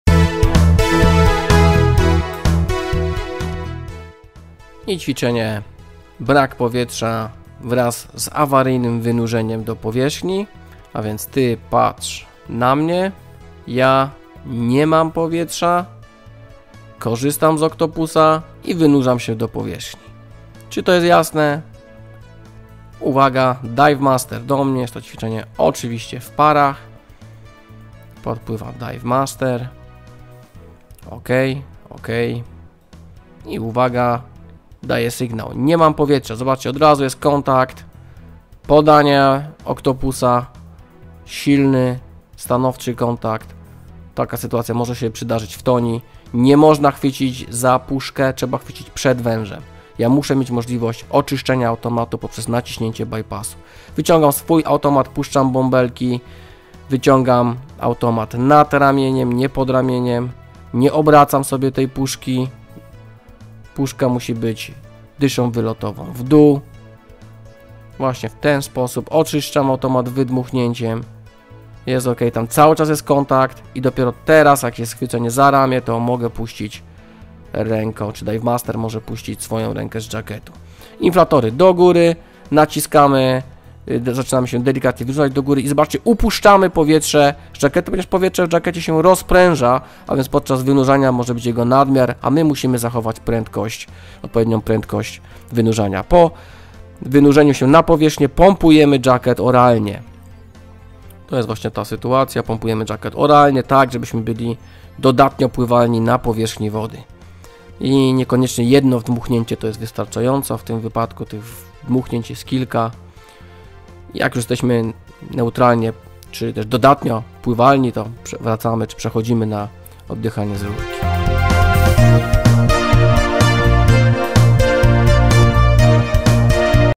17-35. Komentarz; Brak powietrza i awaryjne wynurzenie
Celem ćwiczenia jest symulowanie sytuacji braku powietrza, skorzystanie z pomocy partnera i awaryjne wynurzenie zespołu do powierzchni.

Ze względu na liczne procedury bezpieczeństwa obowiązujące w nurkowaniu oraz dobry poziom przeprowadzanych szkoleń, sytuacja braku powietrza pod wodą jest mało prawdopodobna.

Dzięki temu ćwiczeniu przećwiczysz procedurę właściwego partnerskiego wynurzenia do powierzchni w bardzo mało prawdopodobnym przypadku braku powietrza u jednego z nurków.